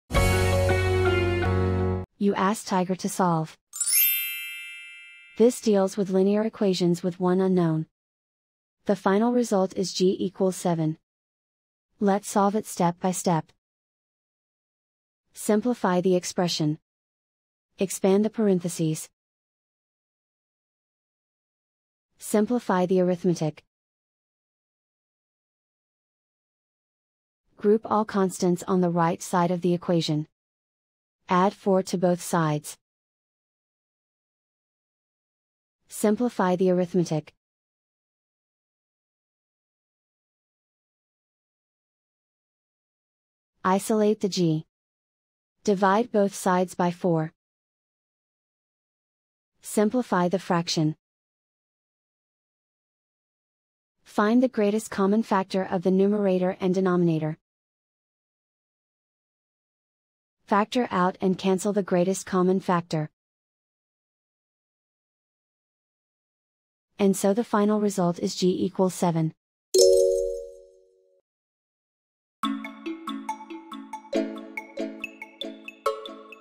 Linear equation with one unknown: Solve 4(g-1)=24 step-by-step solution
Solving 4(g-1)=24 using the linear equation with one unknown solver

for a step-by-step and more tips and tricks for dealing with linear equations with one unknown.